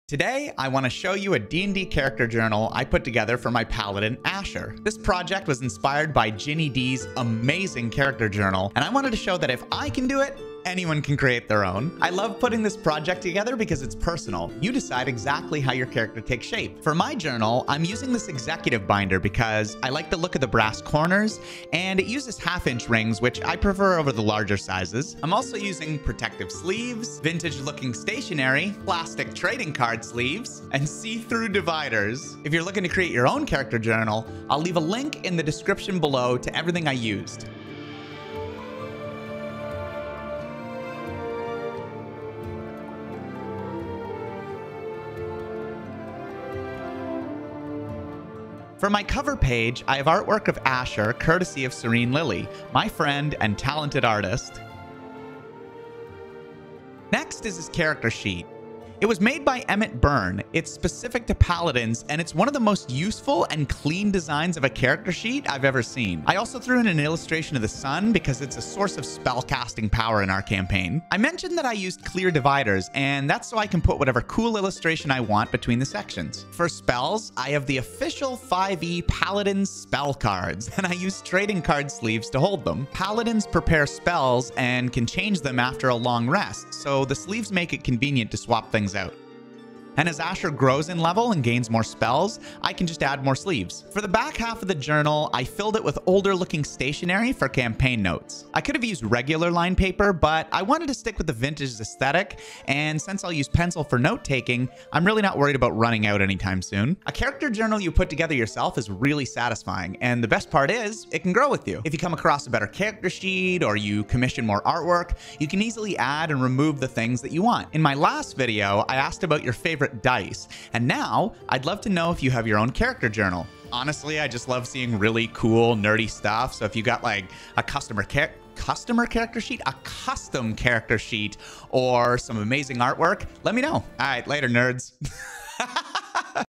I made a D&D Character Journal, Ginny Di style!
After seeing the D&D character journal that Ginny Di put together, I wanted to try out her style and create my own. I'm pretty happy with how it turned out and since I'm not the best at crafts... if I can create a character journal, you can too!

▼ Journal Materials ▼

Protective sleeves
Card sleeves
Notebook paper
Clear dividers (The ones I used aren't in stock but these might be better since you can put your title page right inside them)

▼ Resource Credits ▼

▼ Spot Me On Social ▼

▼ Chapters ▼

0:00 Inspiration
0:17 Materials used
0:37 Inside the journal
1:46 Show me your cool stuff